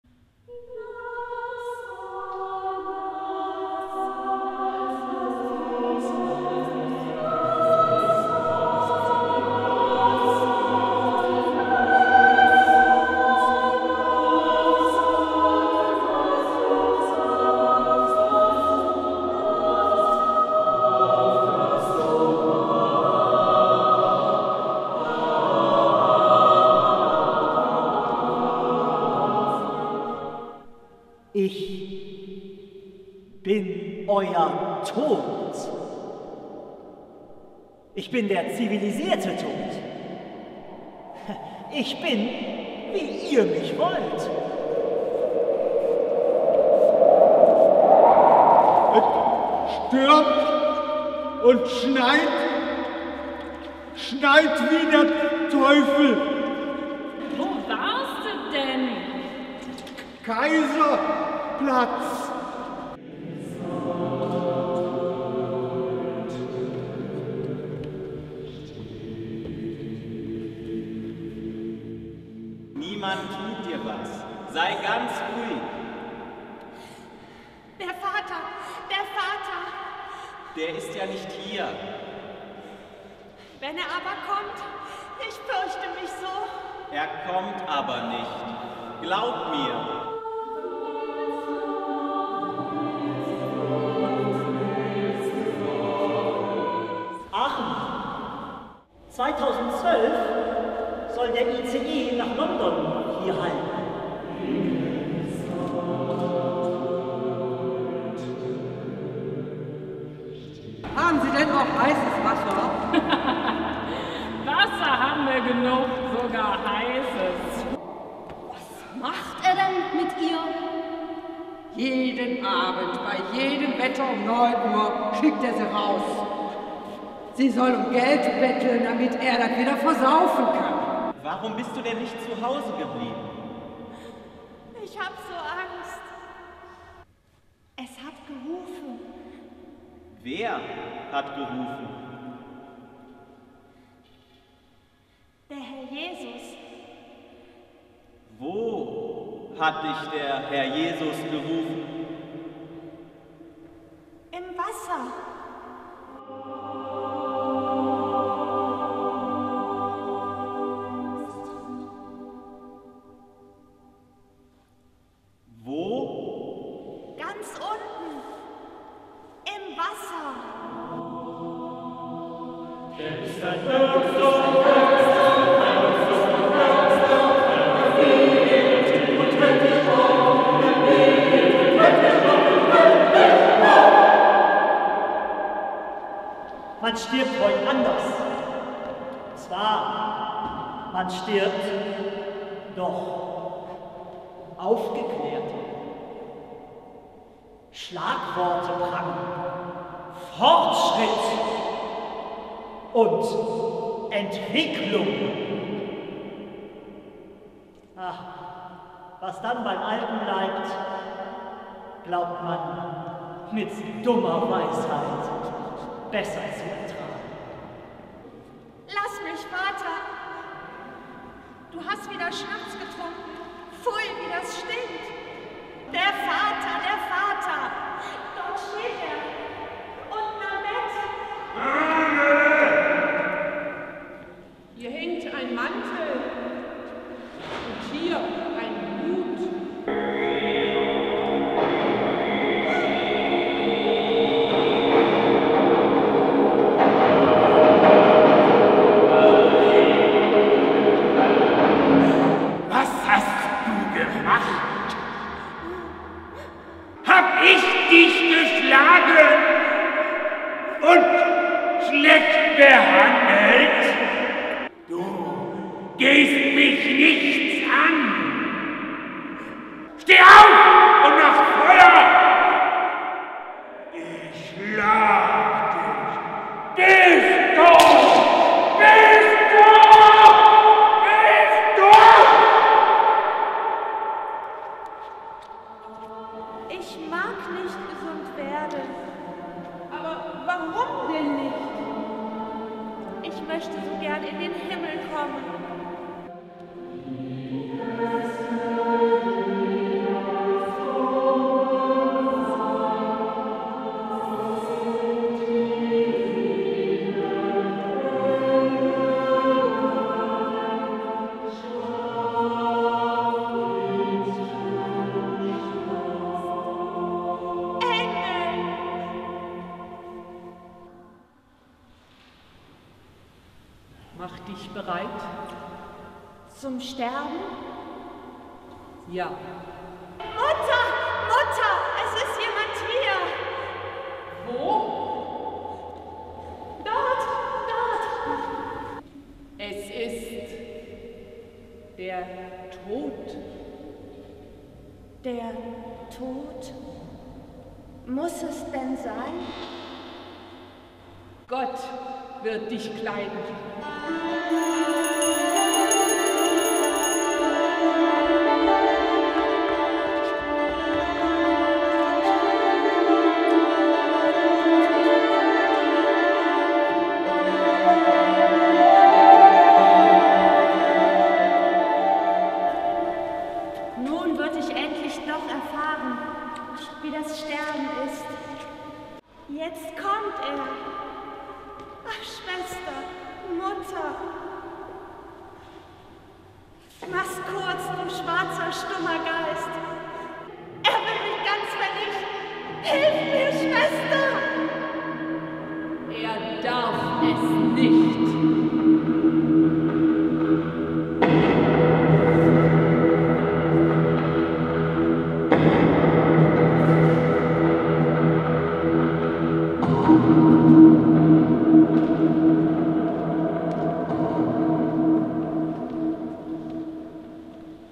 Hanneles Himmelfahrt
Teaser zur Aufführung in der Citykirche in Aachen am Samstag den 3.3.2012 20:00 Uhr
Eintritt frei!

Theater Ludus und das Vokalensemble St. Foillan
Bühnenfassung und Regie: Ingrid Wiederhold
Musikalische Leitung: Kantor Marco Fühner